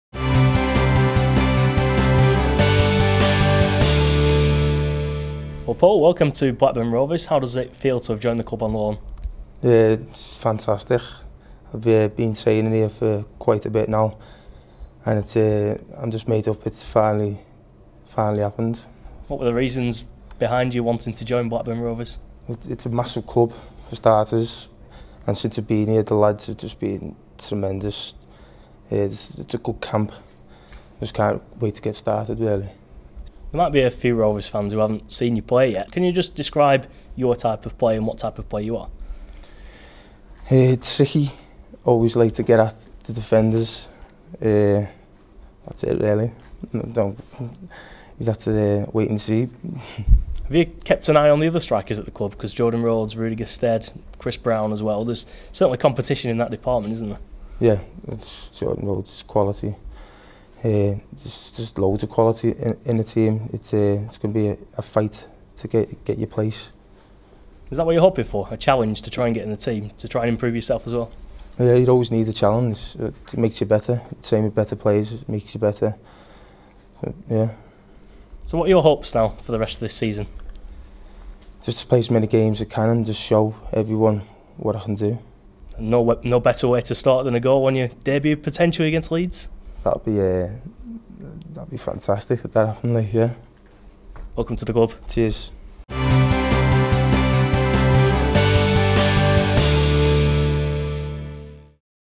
Delighted to join
Blackburn Rovers loan signing Paul Taylor is looking forward to taking his loan opportunity at Ewood and says he has been made to feel extremely welcome at the club and is hoping to get stuck in and impress on the field.